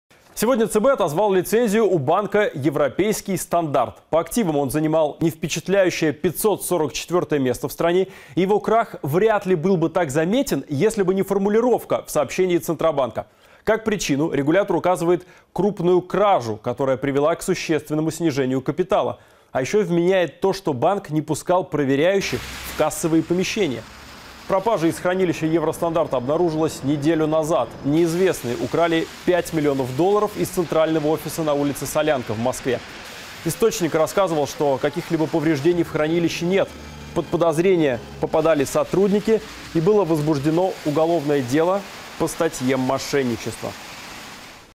Центробанк отозвал лицензии у московского и краснодарского банков
Банк России отозвал лицензии у московского коммерческого банка «Европейский стандарт» и краснодарского коммерческого банка «Новопокровский».